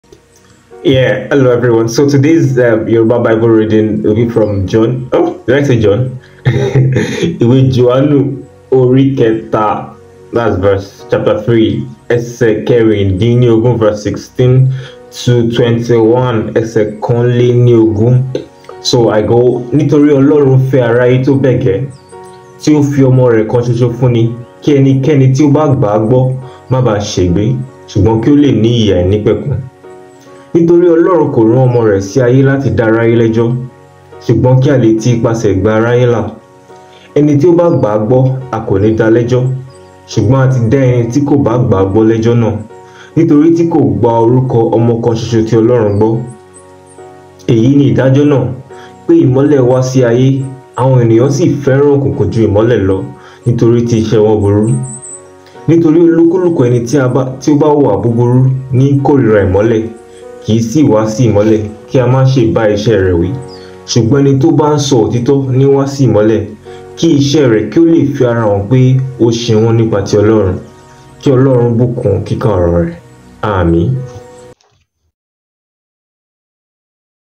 Reading the MOST POPULAR BIBLE VERSE IN THE WORLD in Yoruba language_Yoruba Bible reading_ John 3 16
Reading the word of God in one of my favourite languages